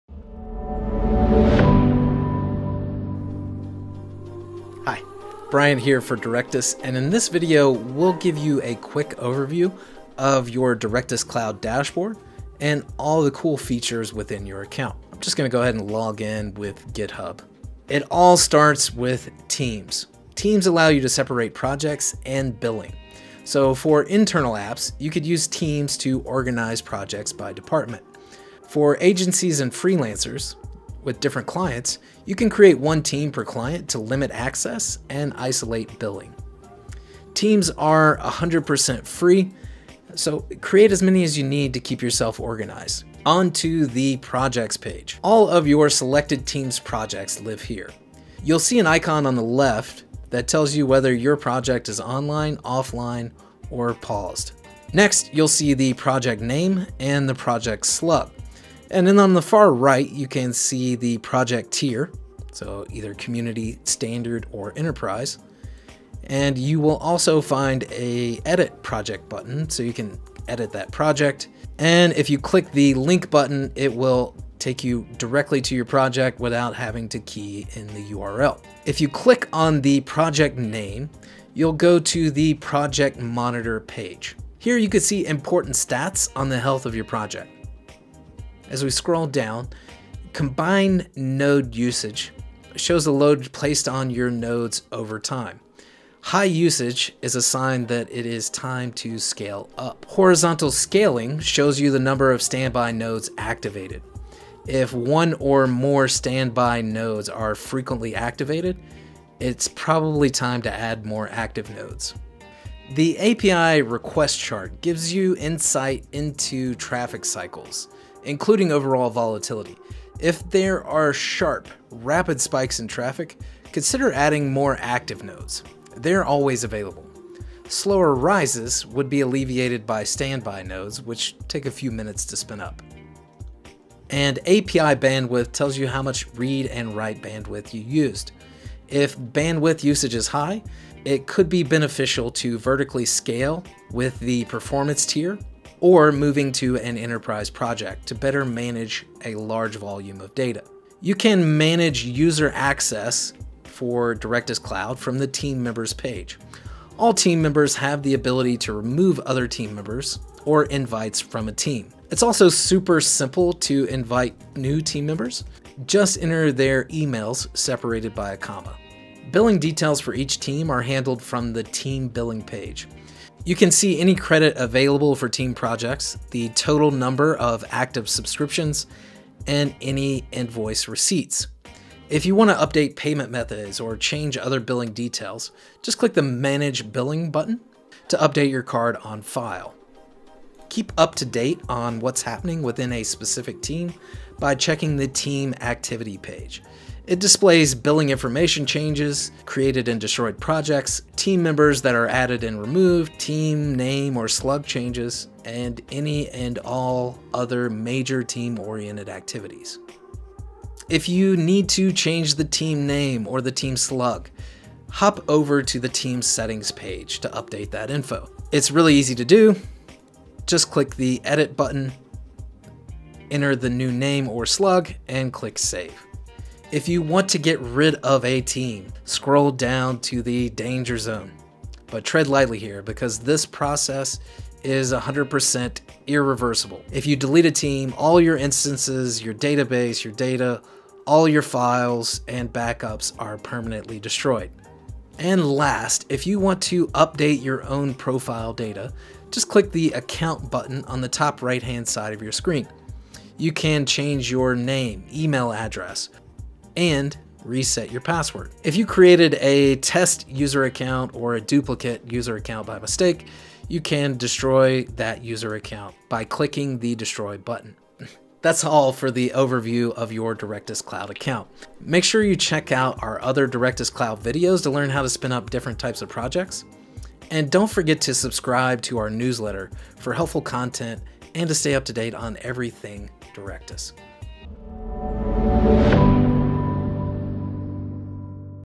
Directus Cloud Overview
Directus is an open-source, headless CMS & API for managing custom databases, and now, our open-source spirit takes to the sky with Directus Cloud. Directus Community Cloud, our free tier, boasts zero quotas or limitations and is ideal for proofs-of-concept and hobby projects. When you're ready for more power, our production-ready, pay-as-you-go Standard Cloud is the most powerful and cost-transparent platform on the market!

--- Timestamps ---
0:16 Login with GitHub
0:20 What is a Team
0:29 Agencies and Freelancers
0:37 Teams are Free
0:43 Project Page
0:48 Project Status
0:58 Project Tiers
1:19 Project Monitor Page
1:30 Combined Node Usage
1:40 Horizontal Scaling
1:52 API Requests
2:14 API Bandwidth
2:31 User Access
2:42 Invite New Members
2:49 Billing Details
2:59 Invoices and Receipts
3:02 Change Billing Method
3:11 Team Activity Page
3:30 Change Team Name
3:31 Change Team Slug
3:47 Destroy Team | Danger Zone
4:06 Update Profile
4:13 Reset Password
4:19 Destroy Account